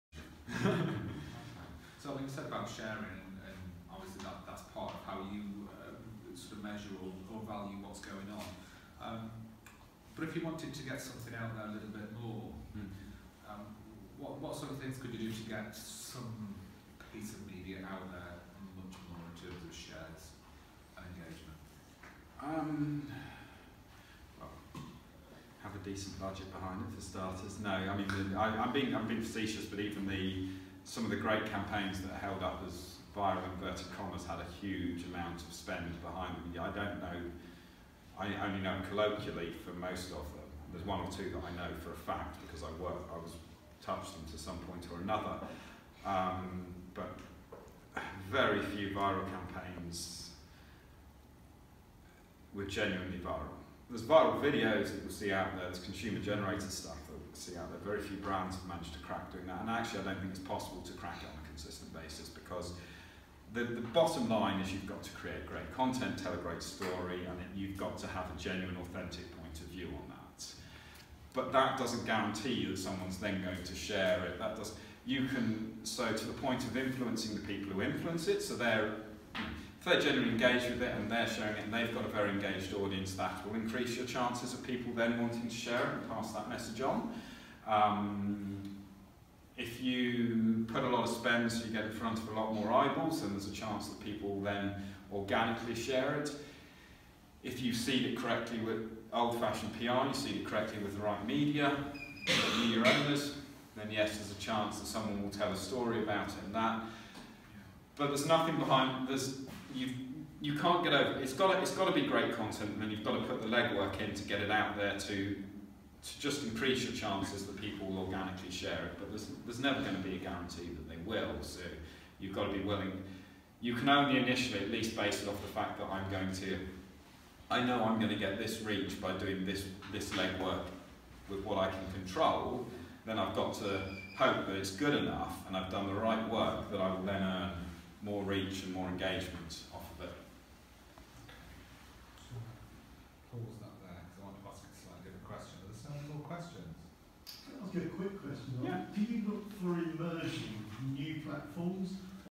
The Marmite Marketer: social media Q&A
A group session Q&A with a member of the Unilever marketing team for Marmite about social media marketing. A section of the session discussing sharing on social media and it's impacts on organisations.